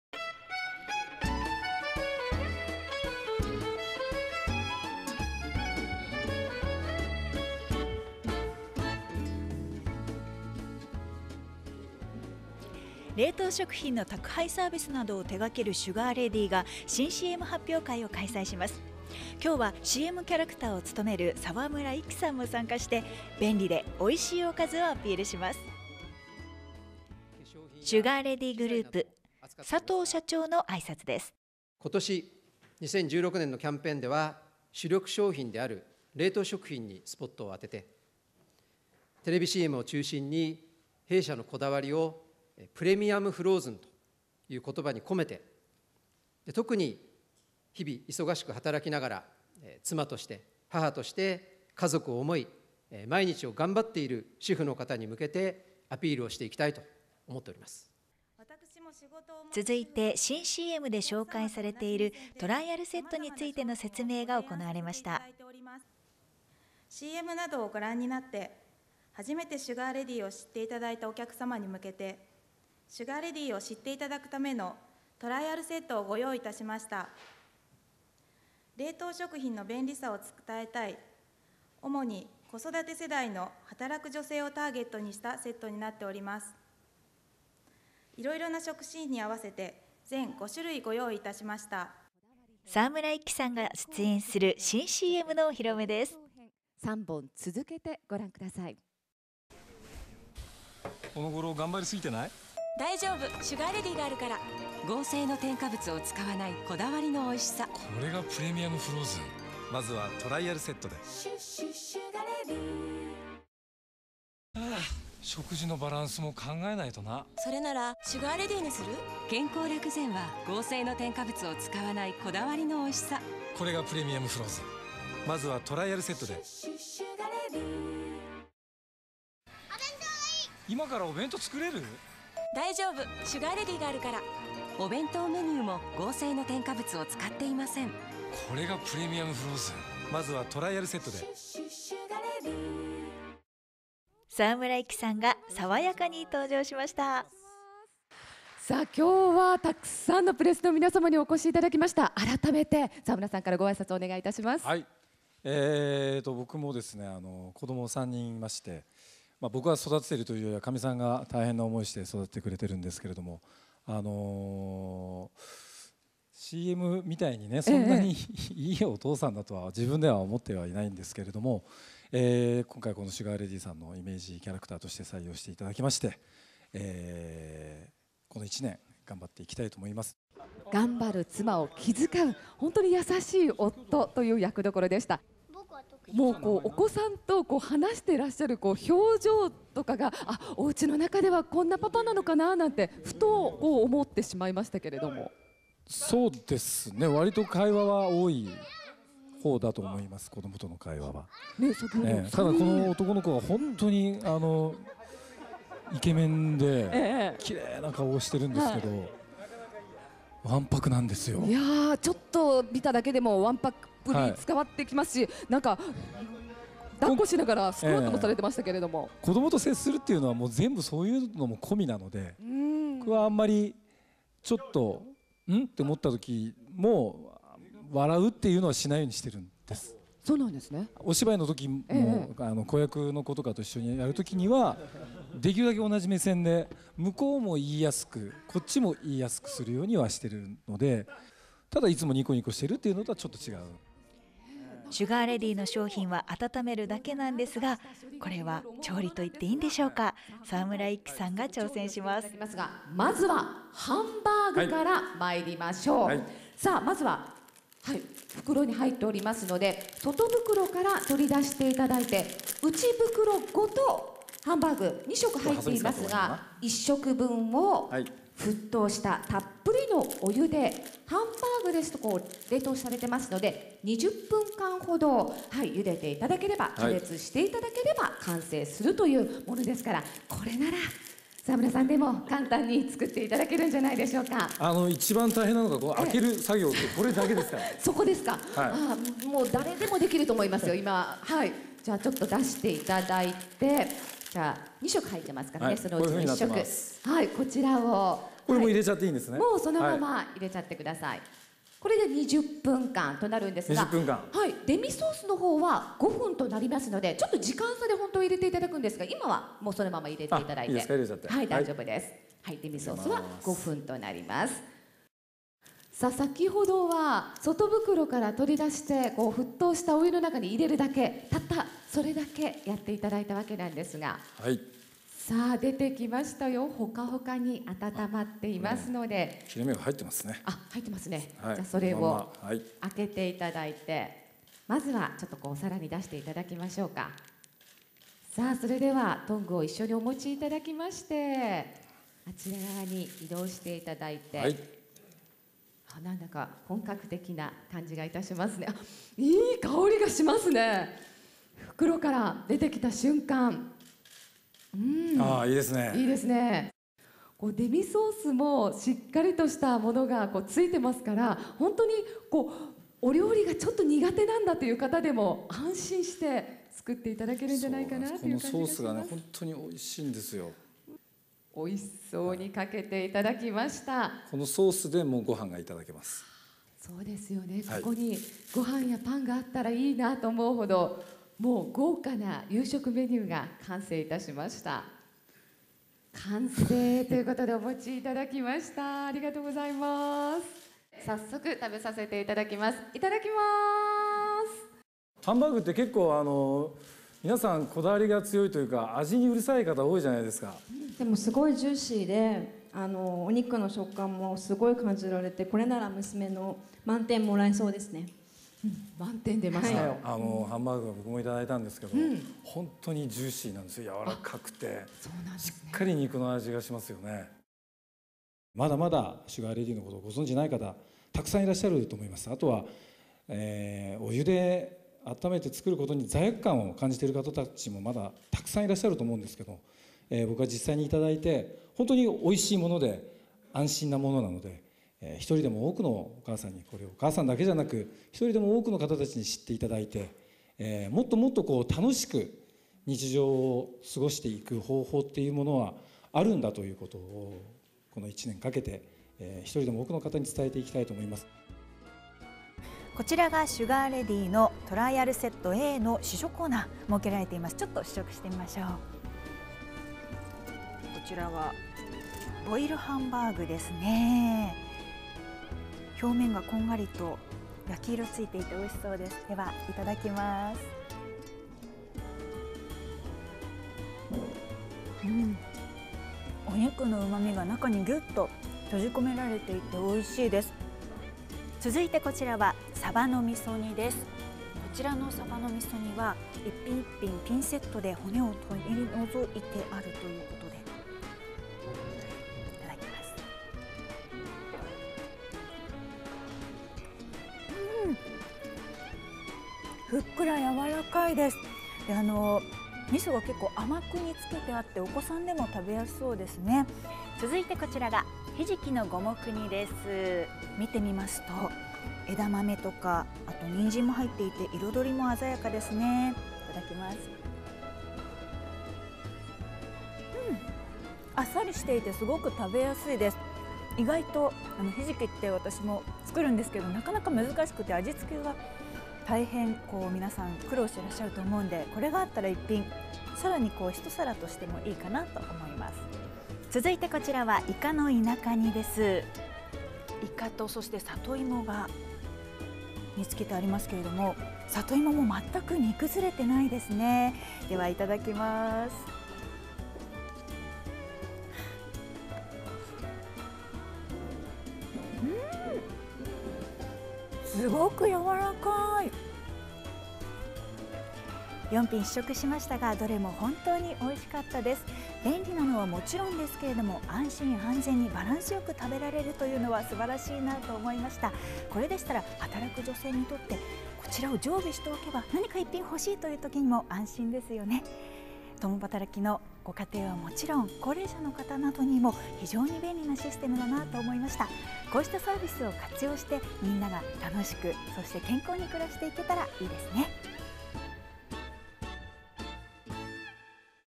沢村一樹さんが料理にチャレンジ？ シュガーレディ新CM発表会
シュガーレディグループは4月5日、沢村一樹さんを起用した新CMの発表会を都内で行った。
　 安全性を追求した加工・冷凍食品、食材、調味料をはじめ、栄養補助食品、キッチン・テーブルウェア、無添加基礎化粧品などの開発・宅配・通販サービスを 行っている同社の佐藤健社長は、主力の冷凍食品を「プレミアム・フローズン」として、頑張っている主婦に向けてアピールしたいと挨拶。
　今年のキャンペーンとして、初めて利用する人向けに4品をパックにしたトライアルセット5種を、定価の2割引で販売すると発表。このトライアルセットをPRする新CMでは、仕事や子育てに忙しい妻をやさしく気遣う夫を、俳優の沢村一樹さんが演じている。
　発表会に登場した沢村さんは「料理は苦手だけど、これならできる」と、実際にステージで商品をボイル。ハンバーグなどの料理を完成させ、「簡単で便利、化学的合成添加物を使っていないので安心だし、何よりおいしい」とアピールした。